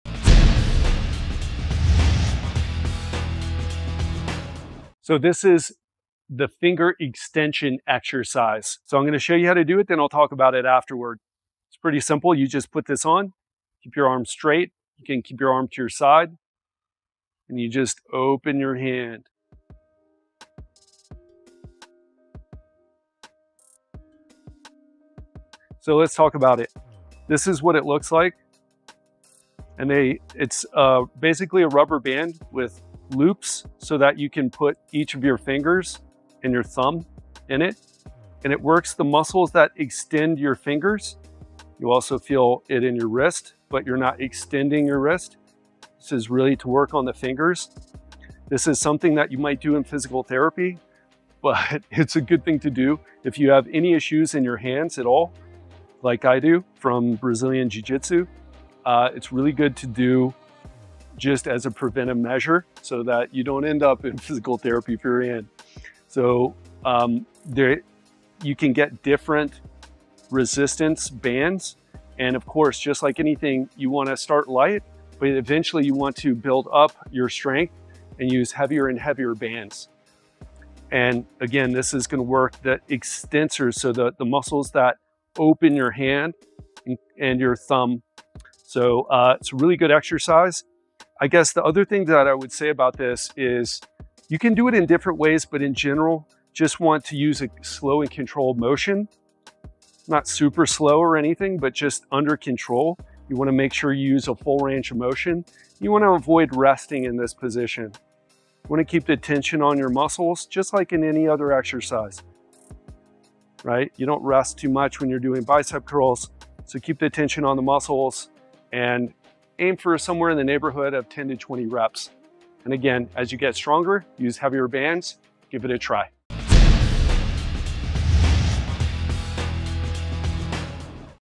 Finger Extension
This video will show you everything you need to know about performing the Finger Extension exercise. Specifically, you'll learn how to do the Finger Extension exercise with a demonstration and explanation from health and fitness expert Ted Ryce.

Instructions:

1. Set Up the Equipment:
   - Use a rubber band with loops or a specialized finger extension band.
   - Choose a band with a resistance level that suits your current strength.

2. Position Yourself:
   - Place each of your fingers and your thumb through the loops of the band.
   - Keep your arm straight by your side or comfortably in front of you.

3. Perform the Finger Extension:
   - Start Position: With the band on your fingers, keep your hand closed, maintaining tension.
   - Open the Hand: Slowly extend your fingers outward against the resistance of the band.
   - Maintain Tension: Avoid fully relaxing the band between repetitions to keep the muscles engaged.
   - Control the Movement: Bring your fingers back to the starting position in a slow, controlled manner.

4. Repetitions and Sets:
   - Aim for 1-3 sets of 10-20 repetitions.
   - Focus on a slow and steady motion to maximize muscle engagement.

5. Form and Technique:
   - Keep your wrist stable during the exercise; the movement should come from your fingers.
   - Maintain a controlled pace—neither too fast nor too slow.
   - Use the full range of motion, extending your fingers completely.

6. Resistance and Progression:
   - Start with a lighter band to get used to the movement and build strength.
   - Gradually increase to heavier bands as your finger extensors become stronger.
   - Use this exercise as a preventive measure to avoid hand injuries or to rehabilitate existing conditions.

7. Usage and Benefits:
   - The Finger Extension exercise strengthens the muscles that extend your fingers, promoting better grip and hand stability.
   - It’s especially beneficial for those recovering from hand injuries, arthritis, or sports that stress the hands, like Brazilian Jiu-Jitsu.
   - Incorporate this into your routine to maintain finger mobility and prevent overuse injuries.

8. Safety Tips:
   - Avoid overextending your fingers or using a band that is too heavy initially.
   - Listen to your body and stop if you experience pain or discomfort.
   - Keep the movement controlled to prevent strain on your fingers and wrist.

Give the Finger Extension exercise a try, ensuring you set up correctly, maintain proper form, and focus on building strength gradually.